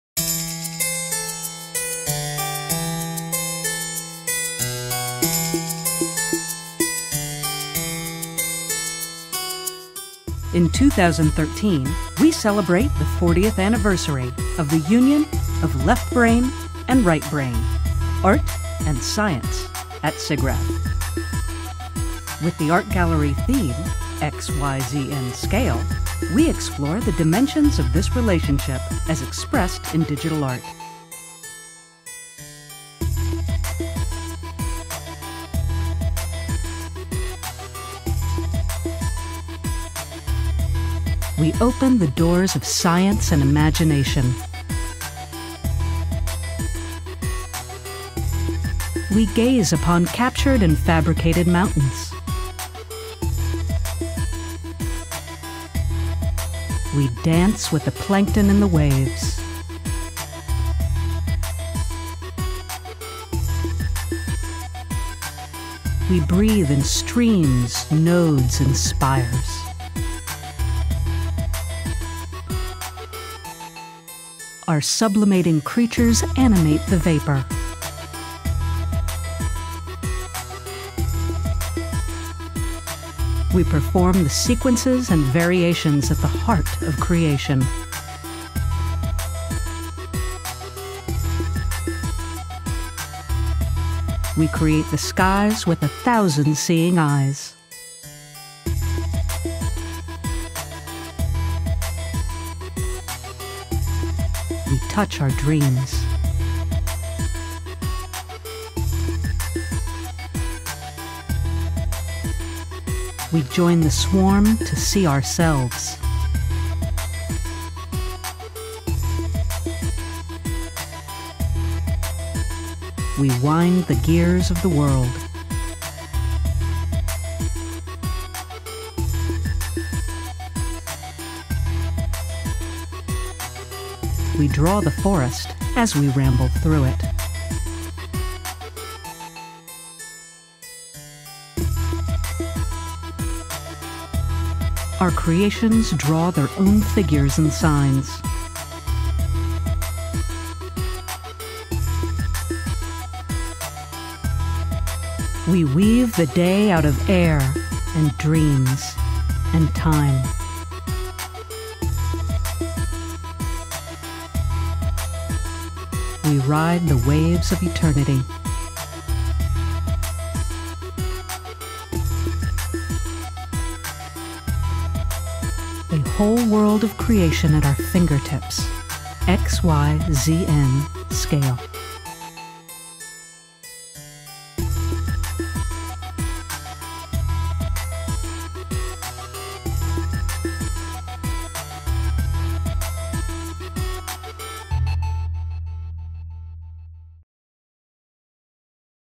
SIGGRAPH 2013: Art Gallery Preview Trailer
The Art Gallery's 2013 theme, XYZN: Scale, draws attention to a key critical affordance of computer-based authorship: the ability to iteratively scale our digital representations at will: in-out-up-down, back and forth, + and -. These core functions enable us to change size and location over time, and at different degrees of resolution. The SIGGRAPH 2013 Art Gallery explores how artists take advantage of these capabilities in the construction of and/or the aesthetic effects created by their work. SIGGRAPH 2013 brings together thousands of computer graphics professionals, 21-25 July 2013, Anaheim, California, USA.